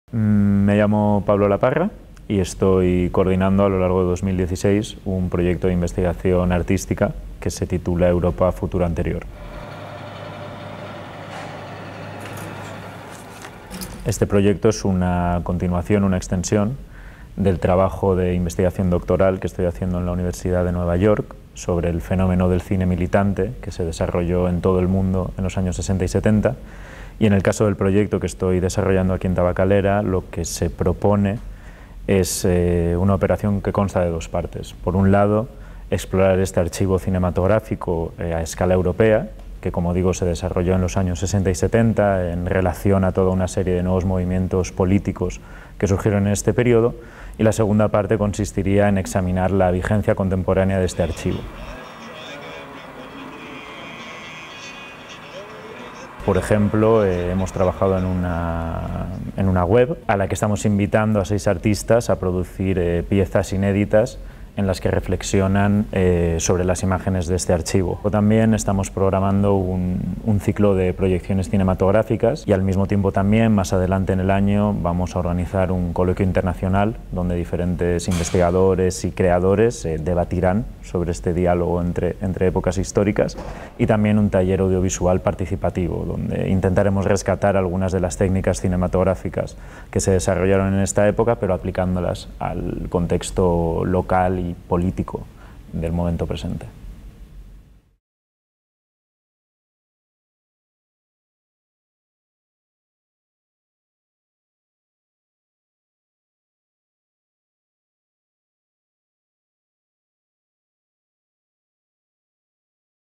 Egoiliarrak | Residentes: Pablo La Parra
Pablo La Parra Tabakaleran garatzen ari den ikerketaren inguruan mintzo da. Europa, geroaldi burutua proiektuak 60 eta 70 hamarkadetako zine militantearen azterketa du oinarria.

Pablo La Parra nos explica las líneas maestras de la investigación artística que está desarrollando en Tabakalera. Europa, futuro anterior parte del análisis del archivo del cine militante de la década de los 60 y 70.
tabakalera.eu/es/europa-futuro-anterior